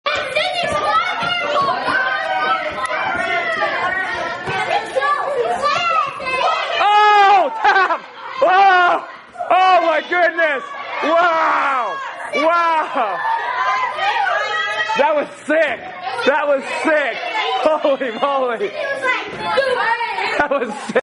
Flying Armbar: Sidney 9 years old hits Sick Flying Armbar
2011 NAGA World Championship Expert Division Champion hits flying armbar during class.  Sidney is 9 years old has only been training for two years and has no formal wrestling or gi brazilian jiu jitsu training.  Wrecking Crew Jiu Jitsu.